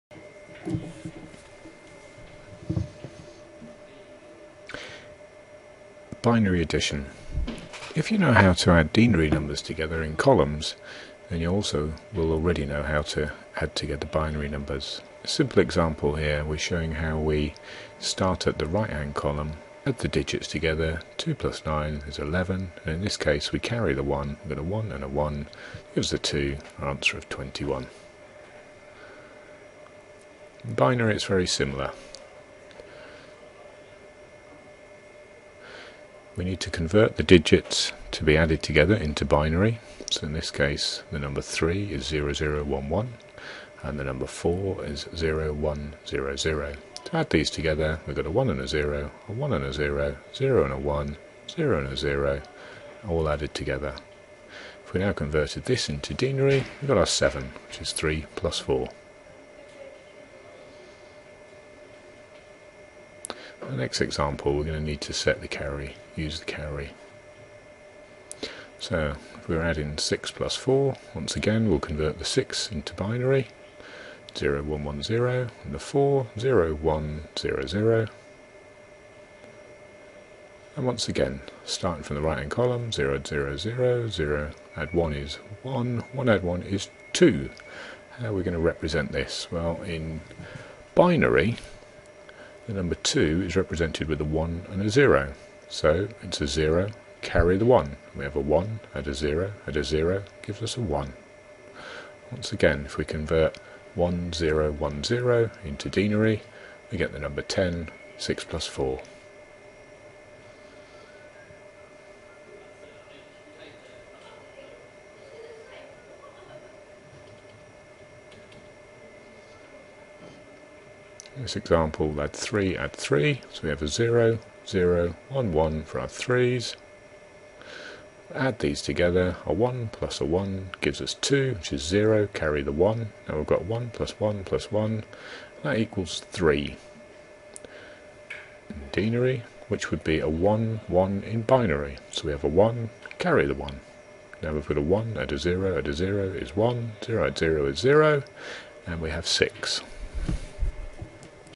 Binary addition
Short video showing how to do addition in binary